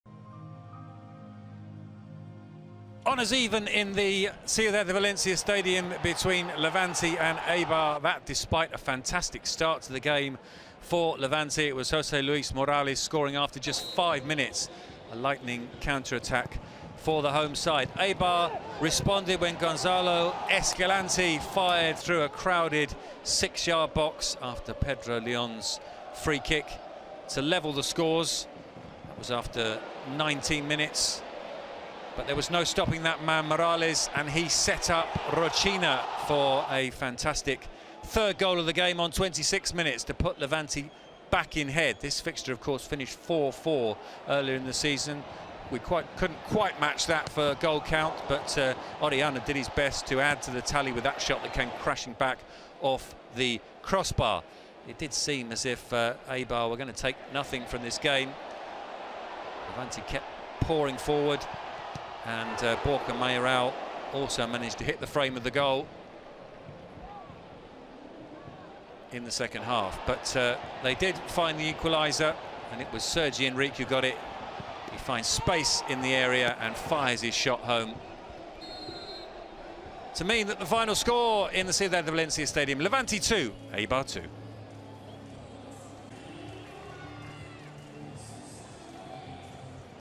Highlights Levante UD vs SD Eibar (2-2)
Levante UD and SD Eibar draw the match 2-2. Morales and Rochina scored for the Paco Lopez's team; Escalante and Sergi Enrich scored for the visitors. LaLiga Santander 2018/2019